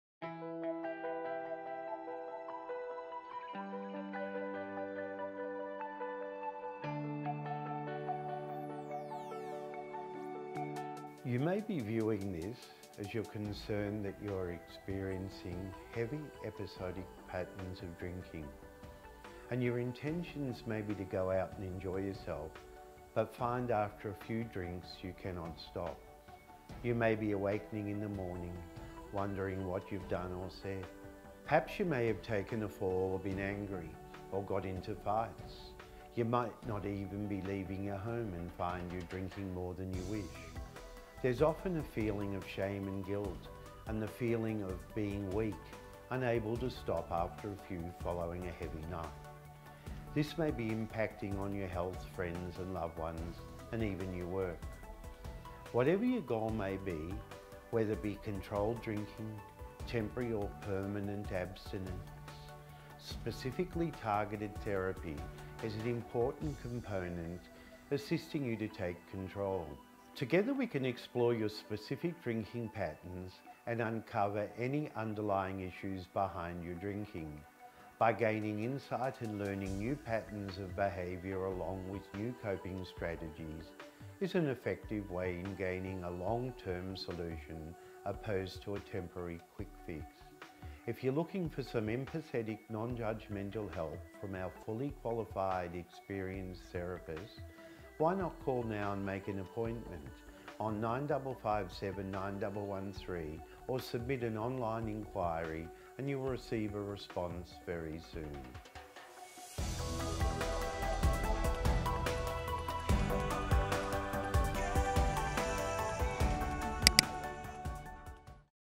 Binge Drinking Treatment
Are you experiencing heavy episodic patterns of drinking? After a few drinks you cannot stop drinking. You may awaken in the morning wondering what you’ve done or said.